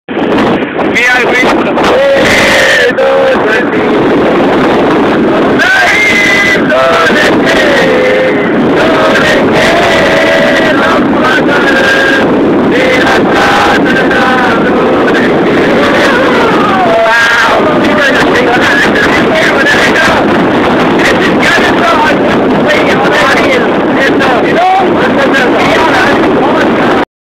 Gyanedra & Me
Gyanendra & Me are heading toward Trikhandi, a religious place of my home town.